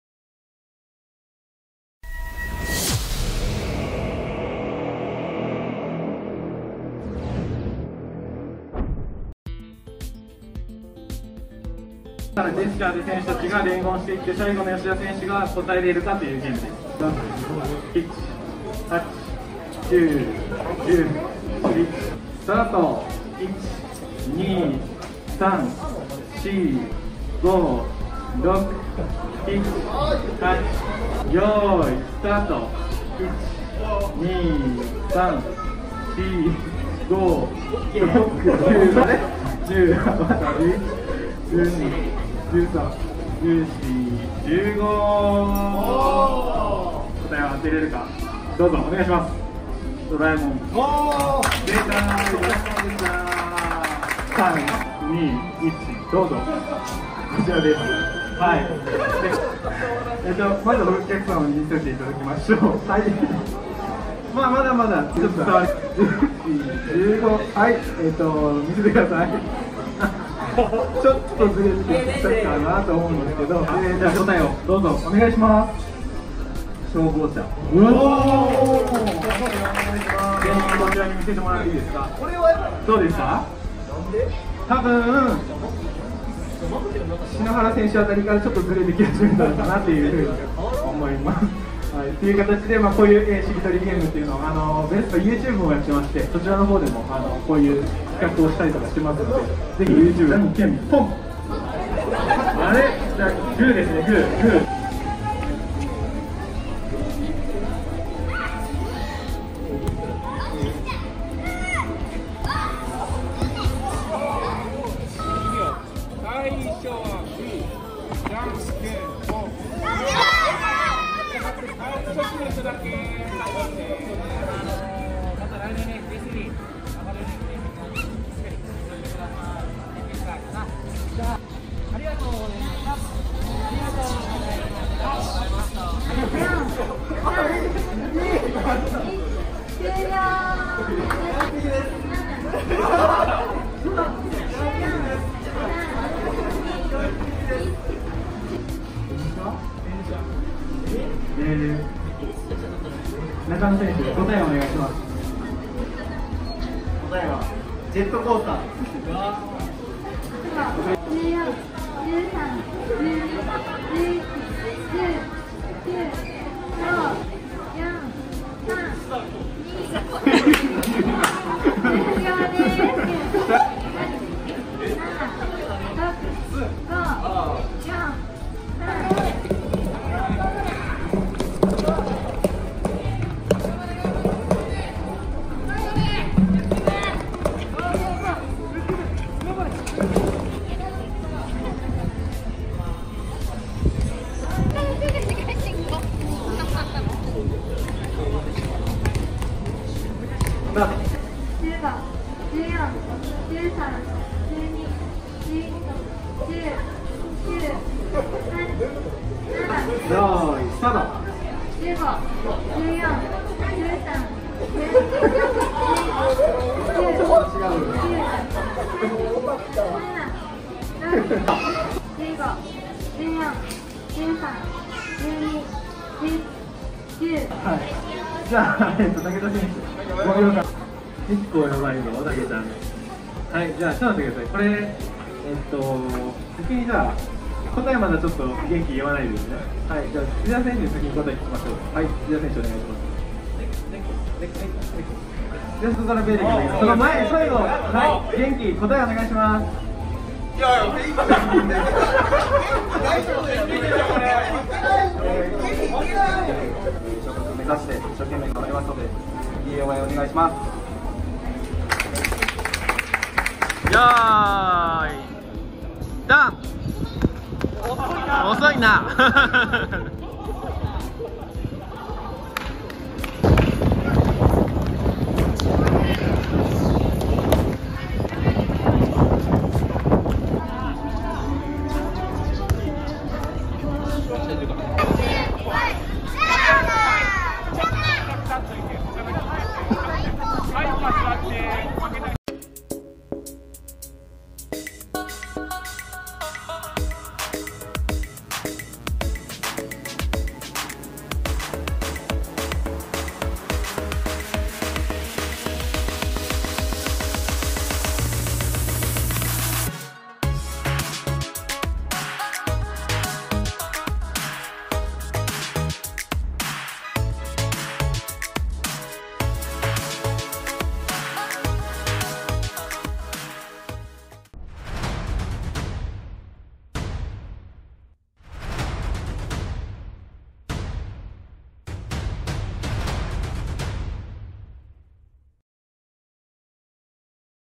【イベント】ファン感謝祭の様子をお届け！
11月23日(水)にアミュプラザおおいたさんにて開催されたファン感謝祭の様子をお届け！
ご来場頂いた皆様ありがとうございました✨